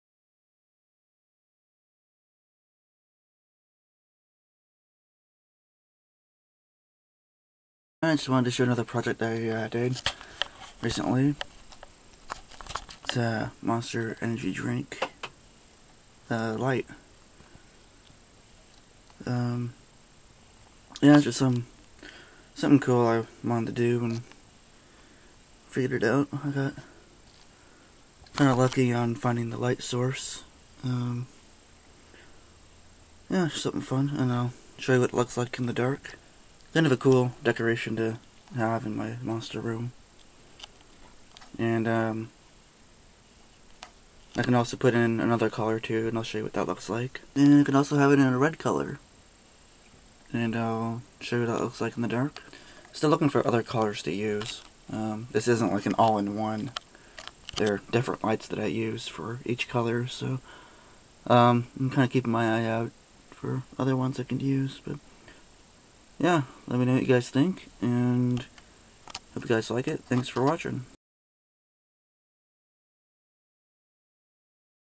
MONSTER ENERGY DRINK-CUSTOM LIGHT
A project idea I had and was determined to figure out. Through a couple lighting trials, I figured it out and am very happy with the results. It took a lot of patience and focus to create it, but it was worth it. I hope you like it and if requested, I can make a How To video of this.